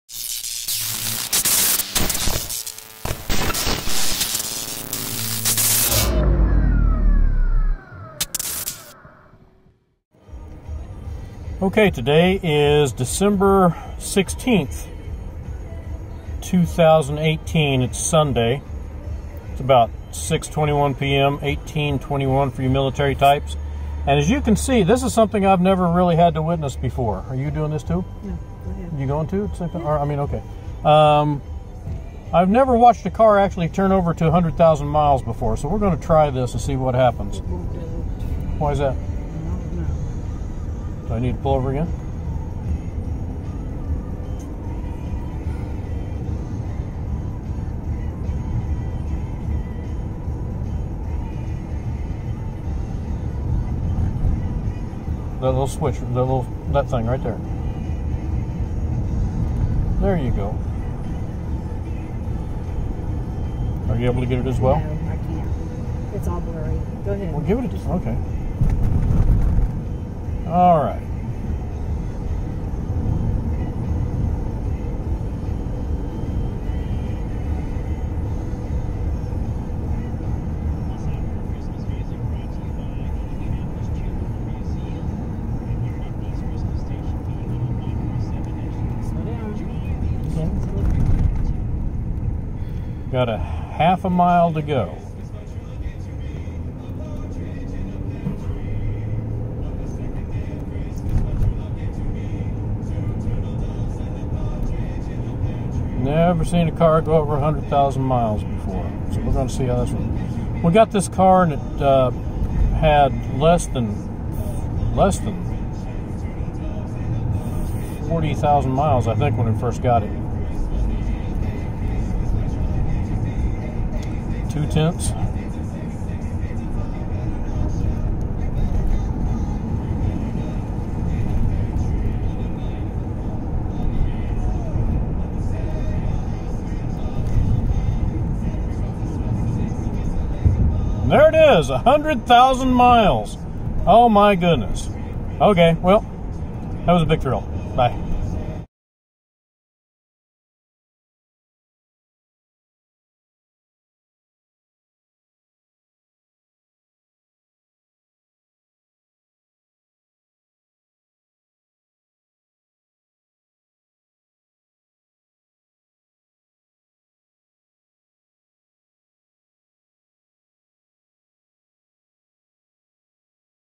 20181216 1821 100,000 miles
December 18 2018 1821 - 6:21pm; 2009 Ford Fusion turning 100,000 miles.